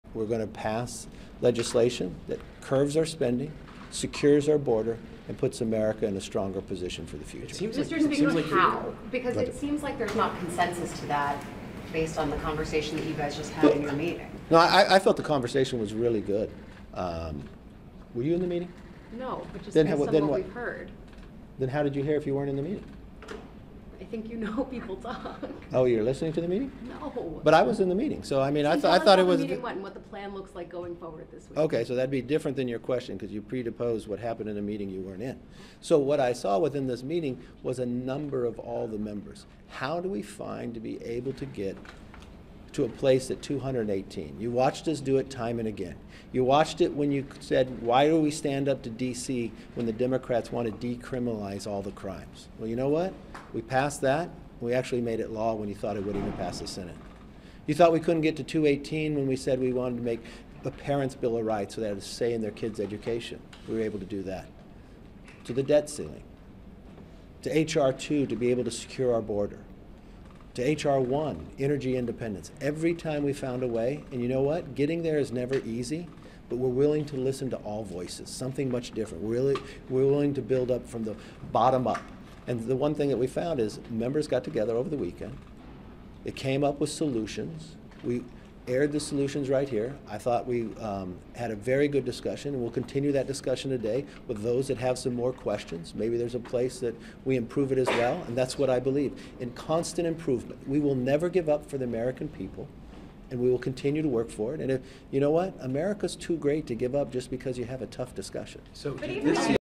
'Were You In The Meeting?': McCarthy Pushes Back At Reporter Asking About Short-Term Funding Bill
At a press briefing yesterday, Speaker McCarthy pushed back on the idea that Republicans would fail to pass a continuing resolution.